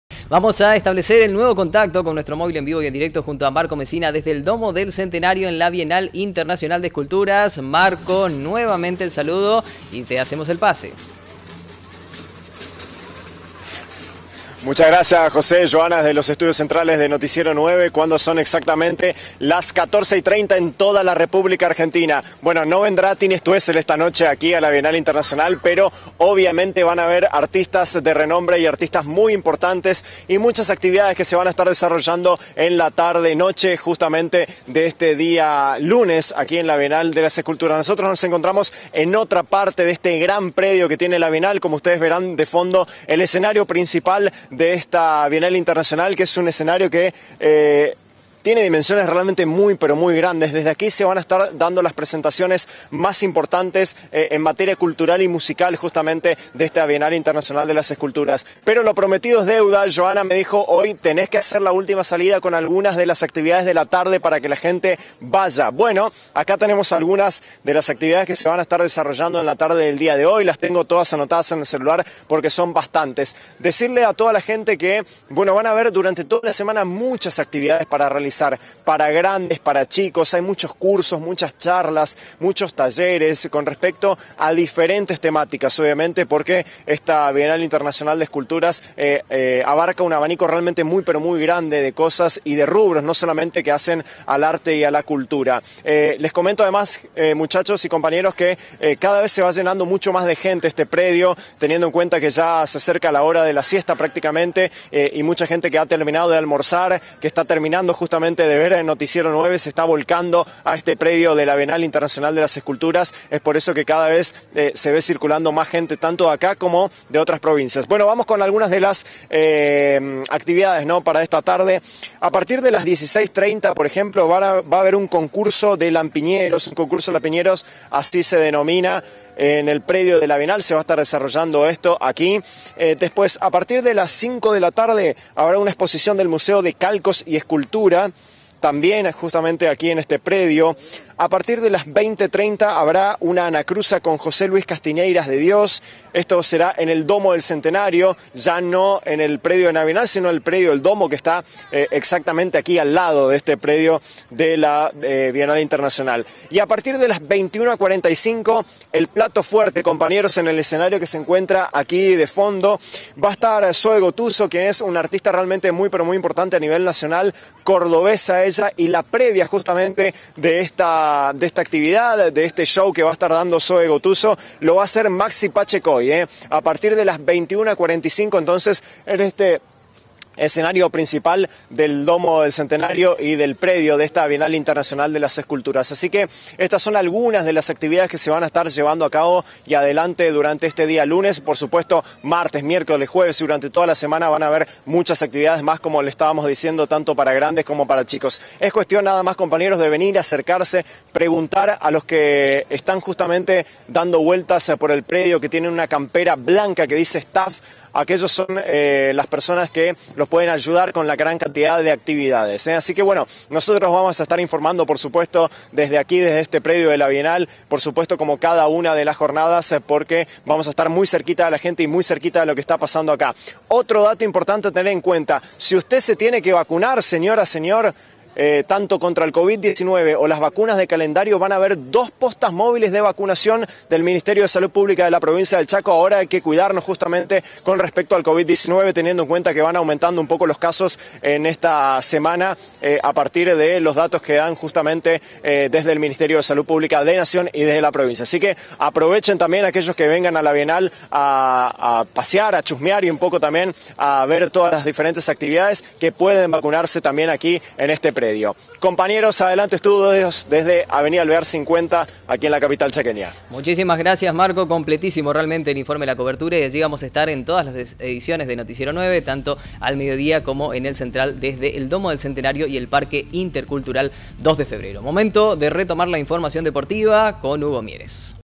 NOTICIERO 9 - LA BIENAL ES UNA FIESTA 1 - MÓVIL EN VIVO
N9 DESDE EL PARQUE INTERCULTURAL 2 DE FEBRERO
El parque intercultural 2 de febrero reunió a miles de visitantes el primer fin de semana de una nueva edición de la Bienal Internacional de Esculturas. móvil en vivo desde la gran fiesta de la cultura chaqueña.